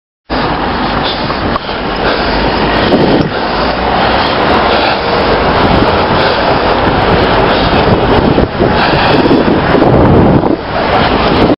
Silly suicidal peds
Crossing on the red man, with me coming, and a van not far up my rear end.